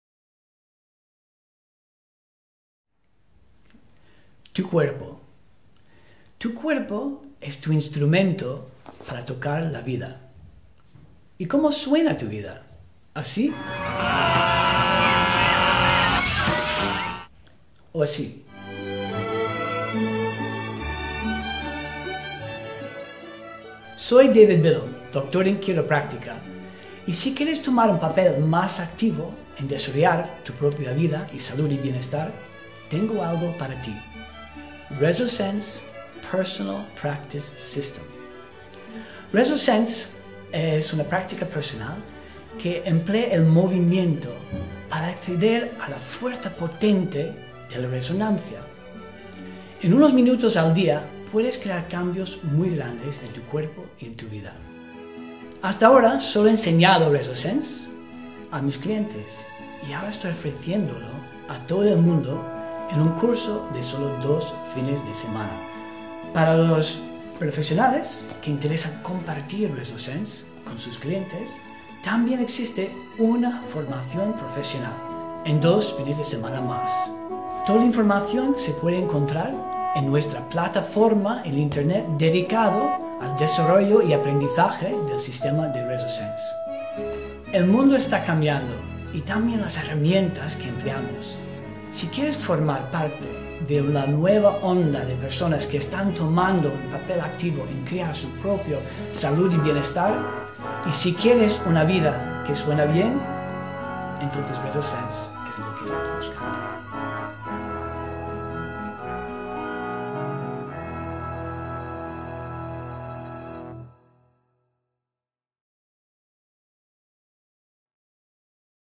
Resosense / David Bilde / Jesús de Frutos / Palma de Mallorca
DAVID BILDE B nos anuncia  brevemente la técnica que ha ideado RESOSENSE,  que sirve para equilibrar su vibración y energía.
Cursos para profesionales y particulares.